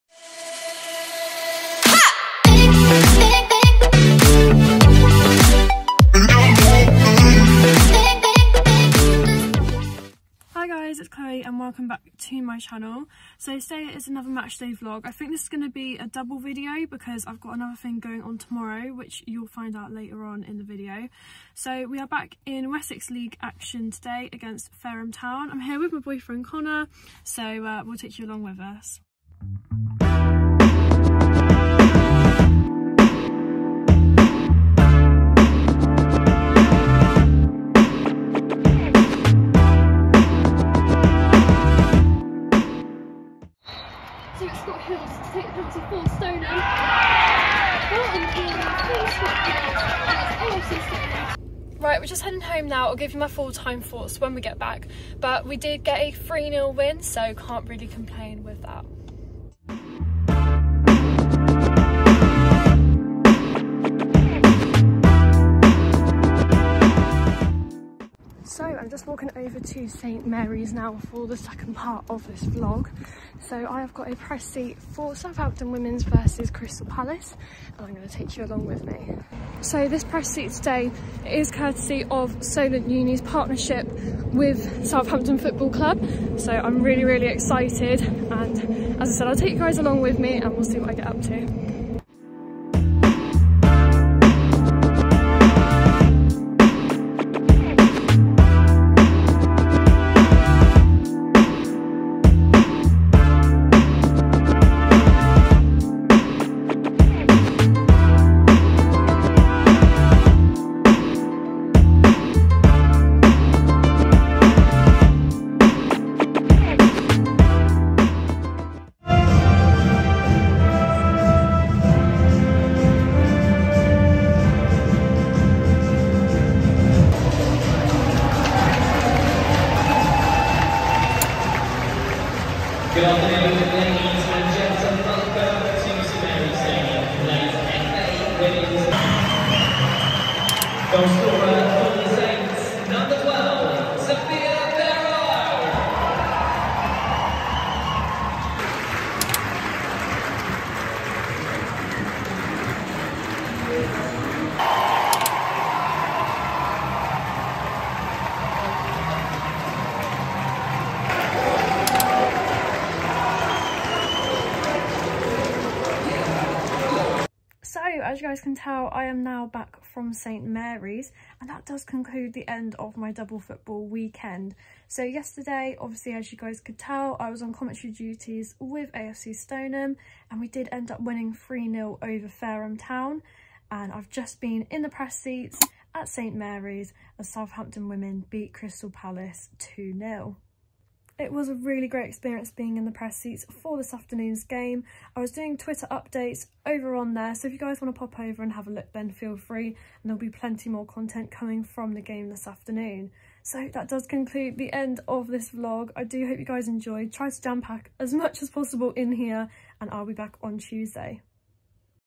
Media Matchday Vlog #2: Press Seat Experience at St.Mary’s + AFC Stoneham vs Fareham Town
Welcome back to another Media Matchday Vlog, this time I brought you along for a double football weekend! Saturday saw AFC Stoneham welcome Fareham Town in the Wessex League before I enjoyed my first press seat experience at St.Mary’s for Southampton FC Women vs Crystal Palace Women!